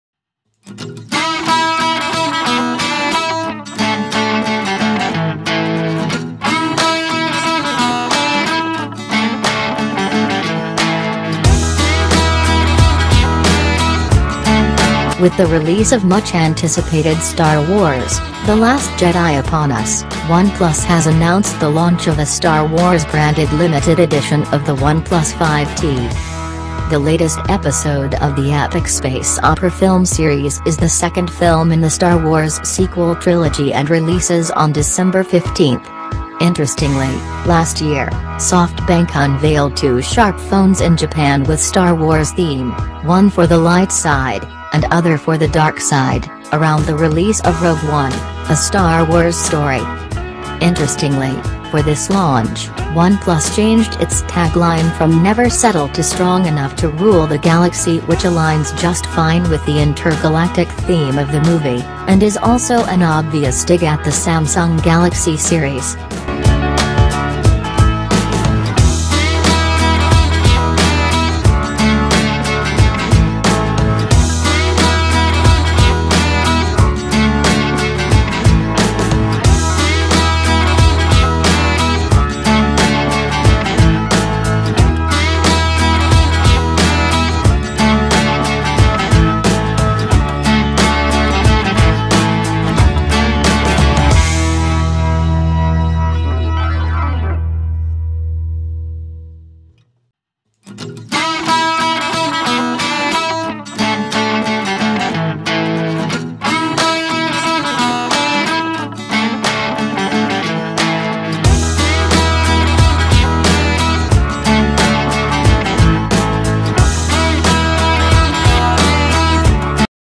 This is the OnePlus 5T Limited Edition - Gadget Life
This is the OnePlus 5T Limited Edition

With the release of much-anticipated Star Wars: The Last Jedi upon us, OnePlus has announced the launch of a Star Wars-branded limited edition of the OnePlus 5T.

The latest episode of the epic space opera film series is the second film in the Star Wars sequel trilogy and releases on December 15. Interestingly, last year, SoftBank unveiled two Sharp phones in Japan with Star Wars theme (one for the light side, and other for the dark side) around the release of Rogue One: A Star Wars Story.

Interestingly, for this launch, OnePlus changed its tagline from ‘Never Settle’ to ‘Strong Enough To Rule The Galaxy’ which aligns just fine with the inter-galactic theme of the movie, and is also an obvious dig at the Samsung Galaxy series.